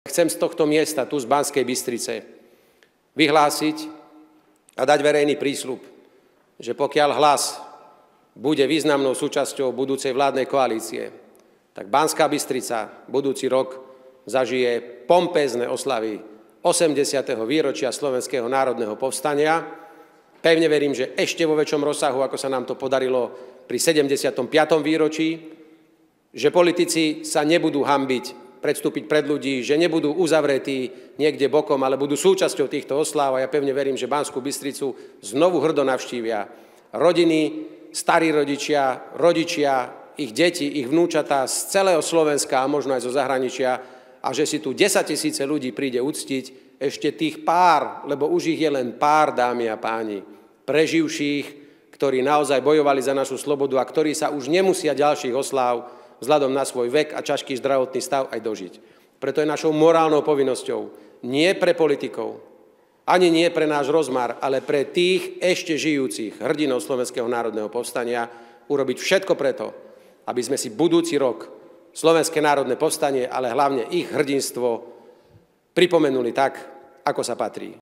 Oslavy SNP dnes vs. oslavy SNP počas mojej vlády
Dnes som dal na oslavách SNP v mojej rodnej Banskej Bystrici veľký verejný prísľub 🇸🇰